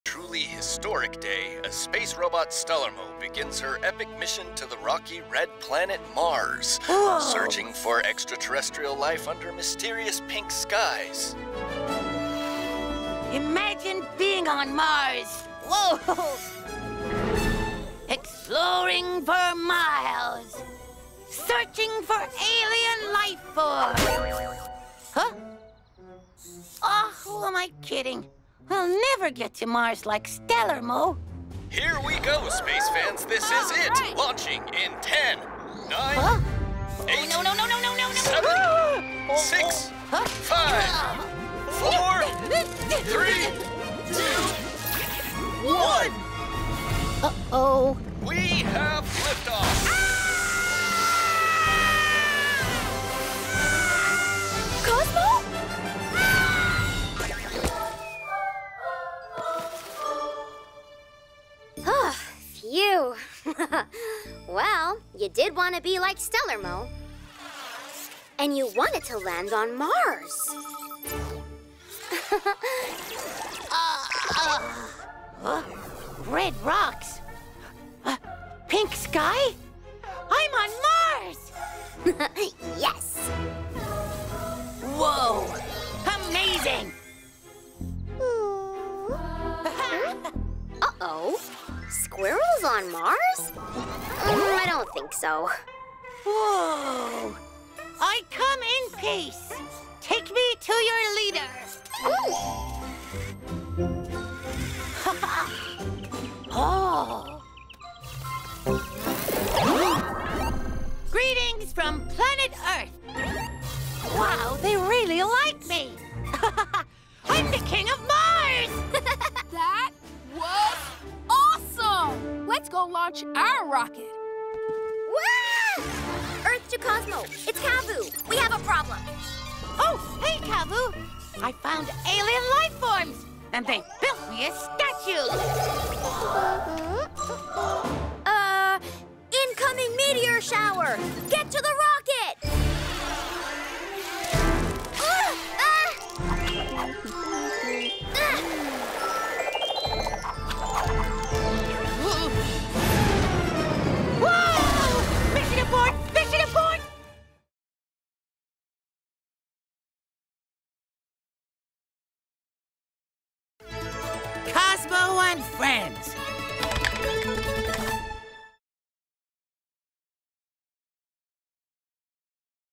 Make your Dreams Come True Day 💭✅ | @CozmoFriends | Awesome Moments | Science for Kids
We are celebrating Make Your Wish Come True Day with Cozmo and Cavu, as she helps his dreams of flying to the planet Mars come true.